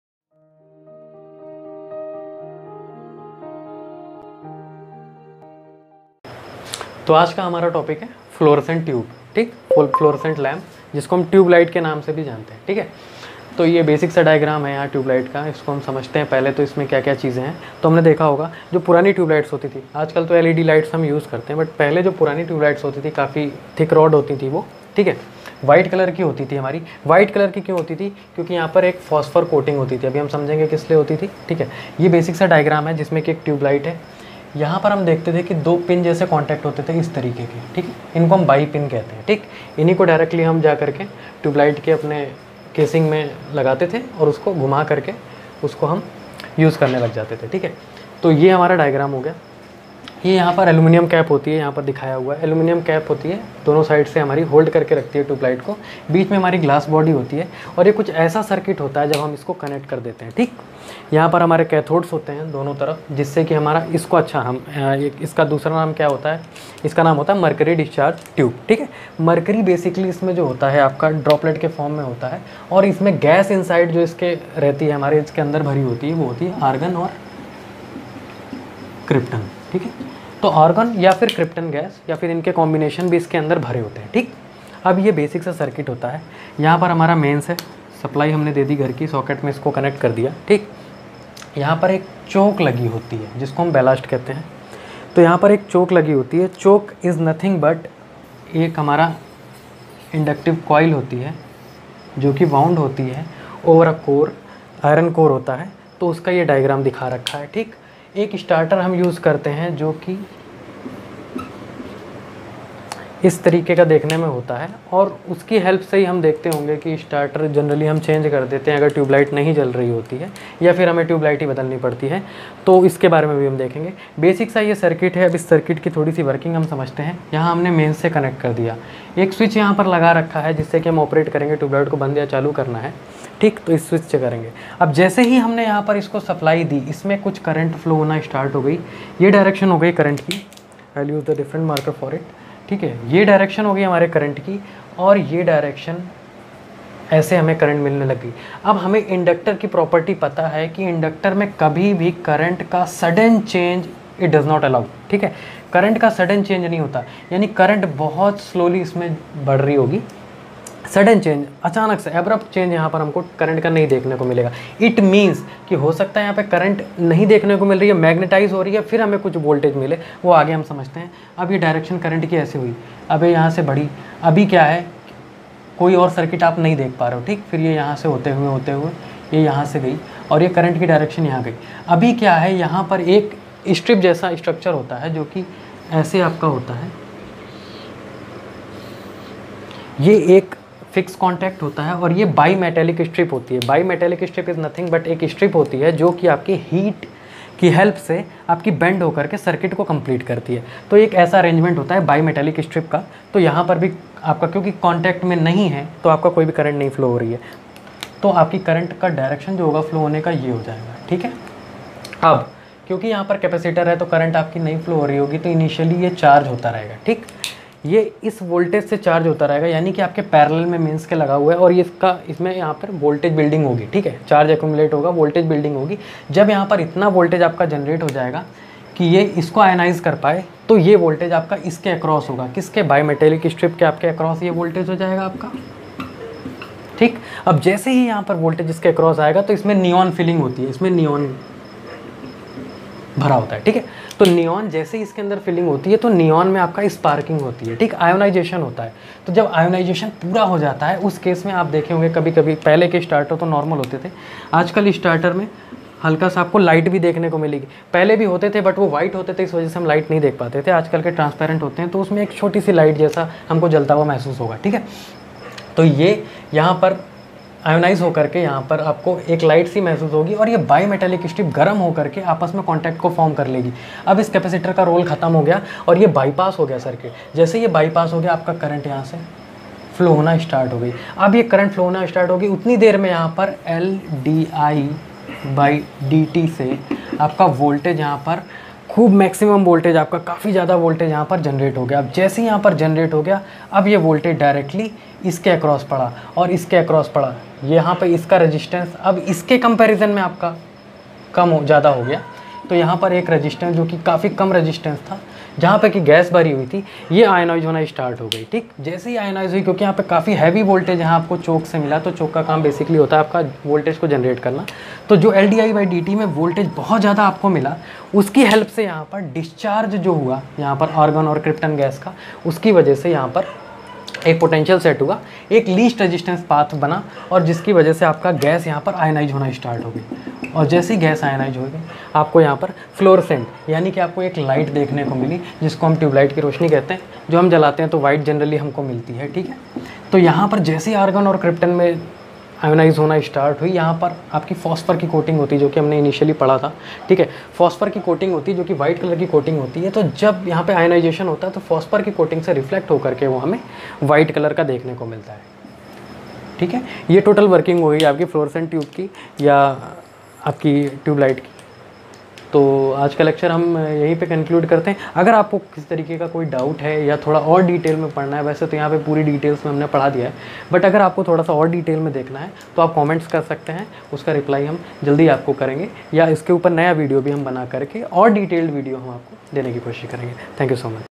Tubelight Working | Choke Coil | Starter | How does a Fluorescent Lamp Work? Working of Tubelight
How a Choke acts in raising up the voltage making ionization of gases possible leading to Fluorescence. Electrical Choke